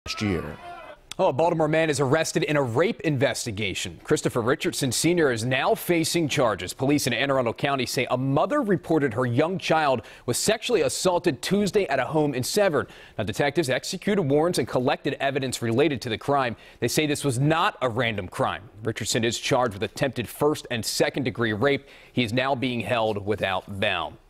Baltimore Man Charged With Attempted Rape Of Young Child In Severn
A Baltimore man has been arrested in a child sex abuse investigation Wednesday.